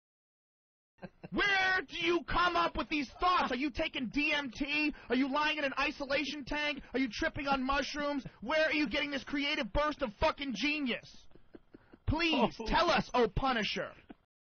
Where do come up with these thoughts? Are you taking DMT?
Joe Rogan on the brilliance of Carlos Mencia.